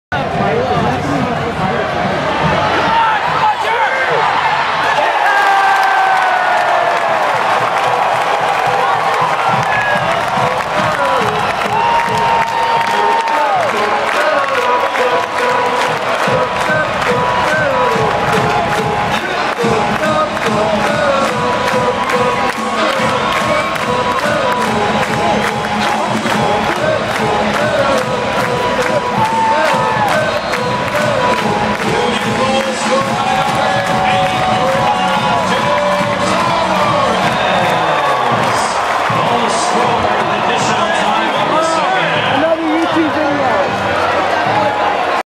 Roger Torres Goal vs Colorado Rapids (7/29/11)
Philadelphia union get a late goal in stoppage time against the defending mls cup champion colorado rapids. but it isnt enough to win the game . union lose 2-1